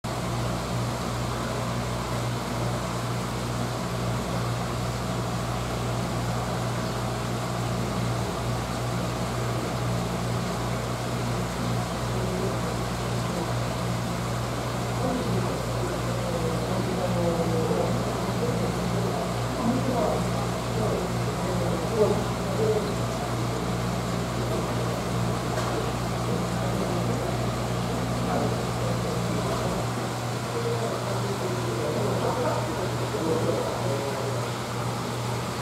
オキアミも食べるウツボ
稀にオキアミでウツボを釣った、という人も。
なるほど、よく食べています（笑）。

The brutal moray which a krill also eats.